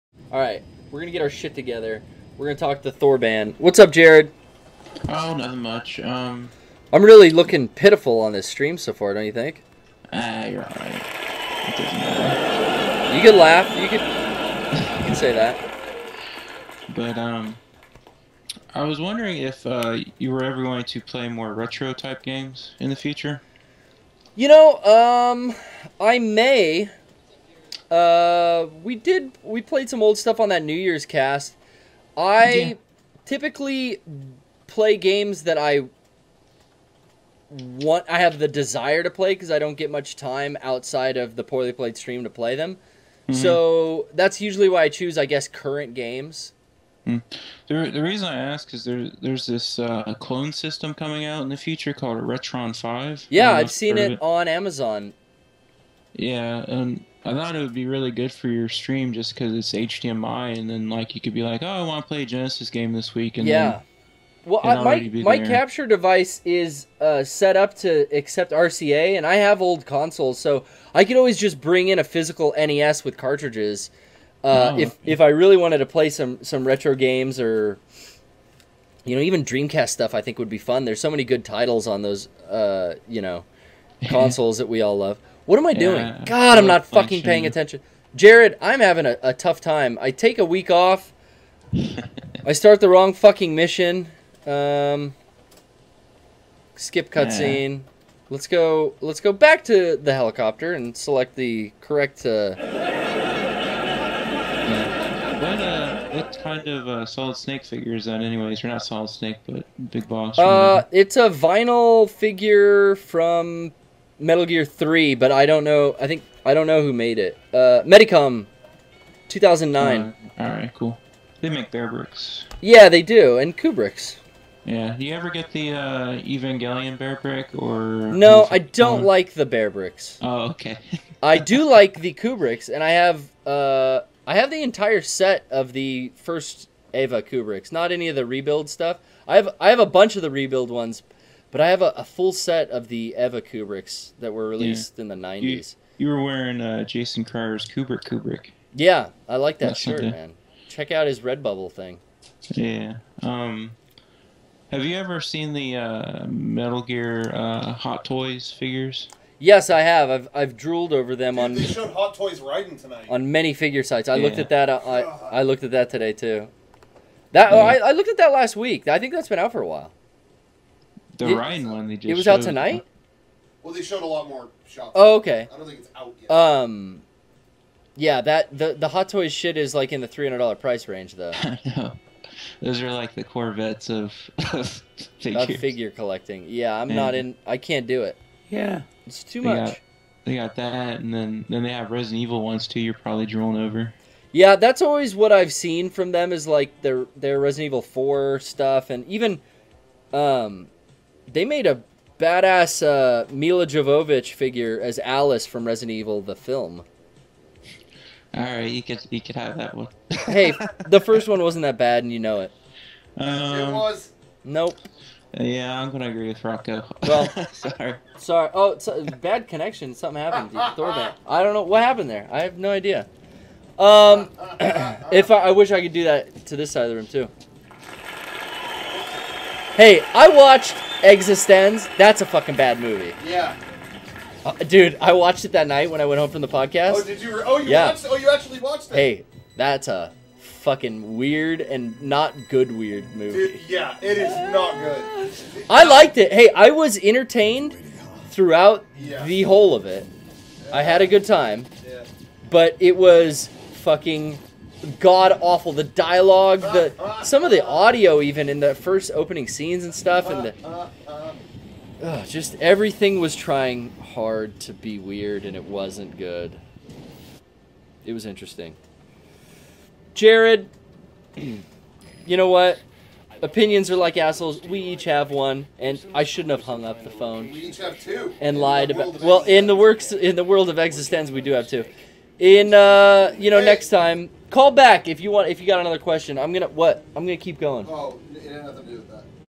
Mega64 Poorly Played Stream 82 - Thorbahn3's Call, Retron 5 & Figures
Jared AKA Thorbahn3 calls back in this week to suggest the Retron 5 to Garrett and to discuss figures.